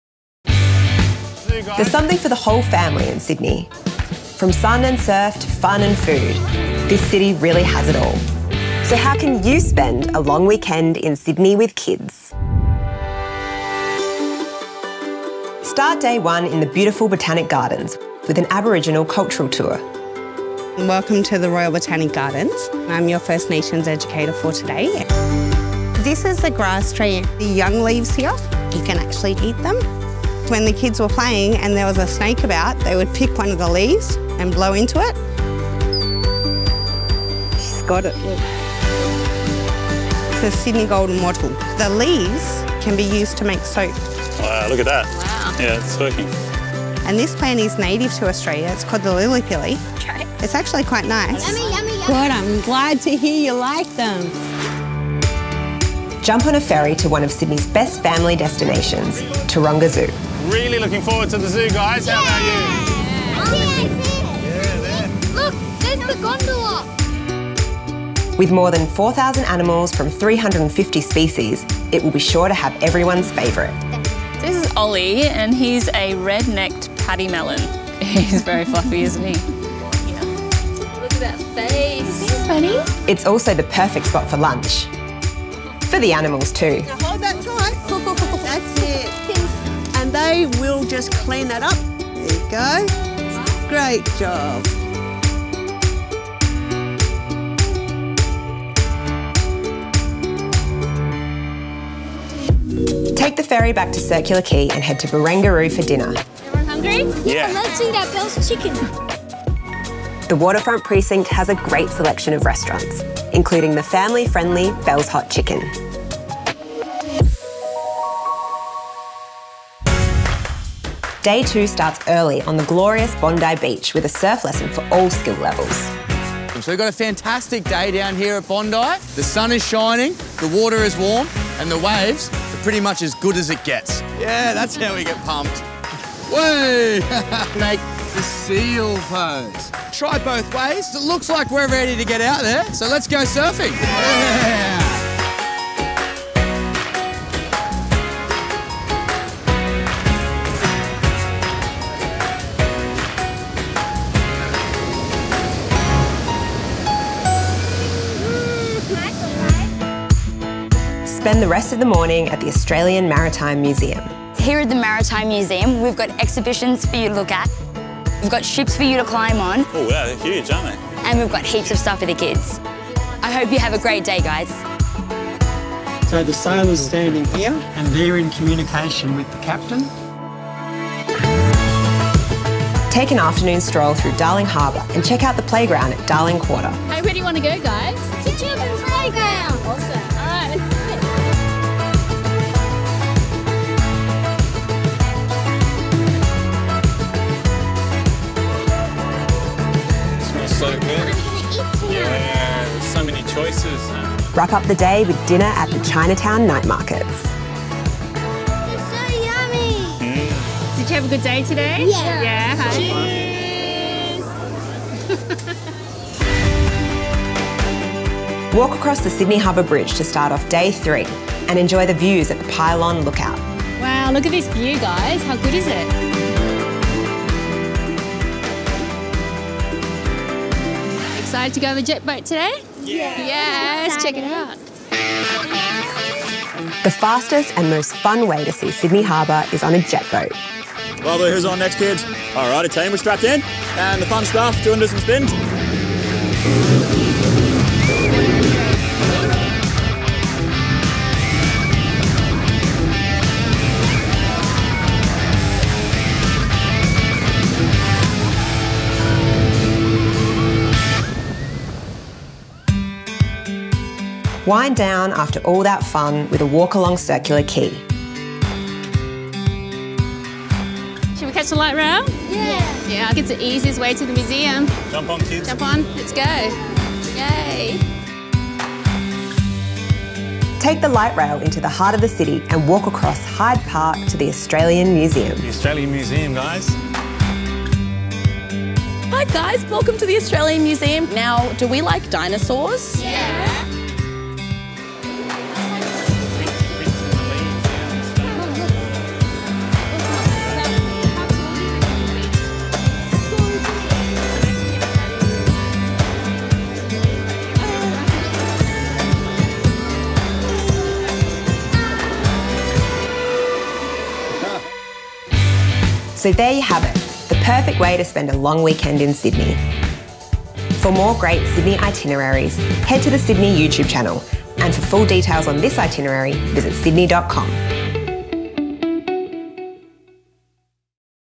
A Long Weekend in Sydney with Kids
Gather your crew and show the kids the best parts of Sydney. Take your pick from surfing lessons to zoo expeditions and ocean pool dips. Here’s your guide to a family-friendly long weekend in the harbour city.

Day 1
00:14 Aboriginal Cultural Tour in the Royal Botanic Gardens
00:57 Ferry from Circular Quay to Taronga Zoo
01:08 Taronga Zoo
01:47 Dinner at Belle's Hot Chicken

Day 2
02:03 Surf lesson on Bondi Beach
02:40 Australian National Maritime Museum
03:14 Darling Quarter Park
03:19 Chinatown Markets

Day 3
03:41 Sydney Harbour Bridge Pylon Lookout
03:54 Thunder Jet Boat
04:32 Tram to The Australian Museum